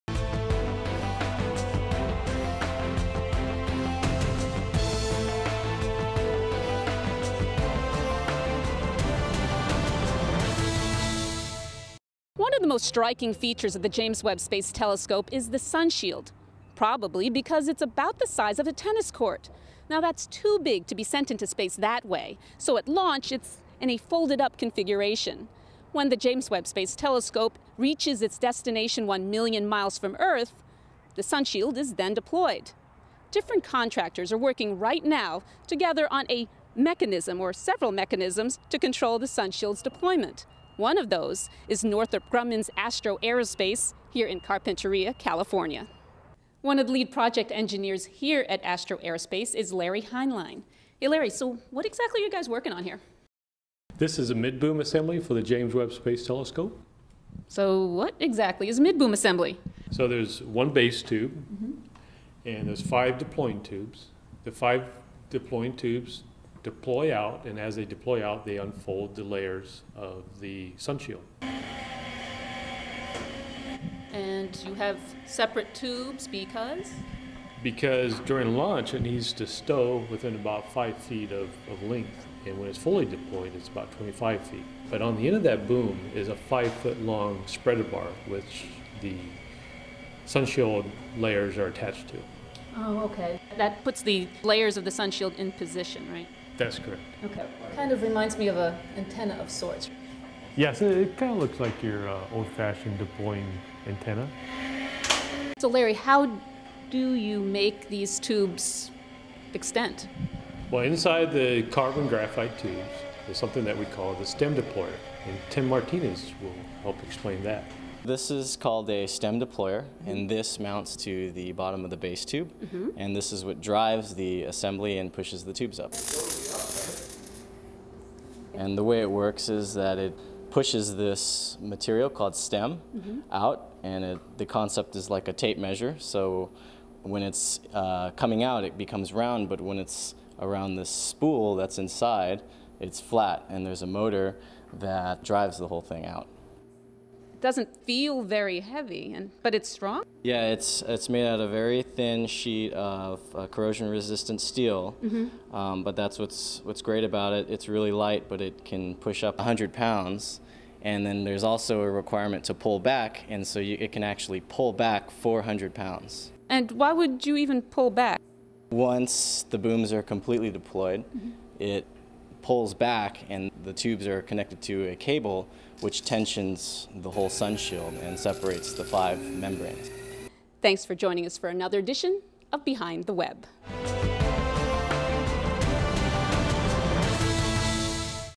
Behind the Webb Stretching Webb's Wings (Episode 8, November 2010)
One of the dominant features of the Webb telescope is its tennis-court-sized sunshield. The sunshield protects the observatory from unwanted light, keeping it cool and allowing it to detect heat from faraway objects in the universe.

Getting such a huge sunshield into orbit, however, is a technical feat. Webb's sunshield–folded up during launch–unraveles in a deployment process as the Webb telescope reaches its destination a million miles from Earth. Two deployable towers, or Mid Boom Assemblies (MBA), serve to stretch the sunshield open. The completion of this hour-long process triggers another mechanism that separates the sunshield's five layers, readying the sunshield for work.

Credit: NASA, Mary Estacion (STScI)